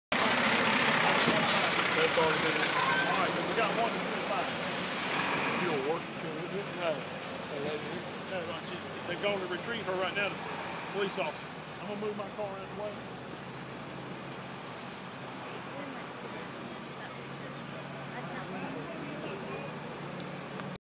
Raw: Train derailment
Wilmington Fire Department crews respond to a train derailment at King and Railroad streets, the entrance to the Love Grove neighborhood.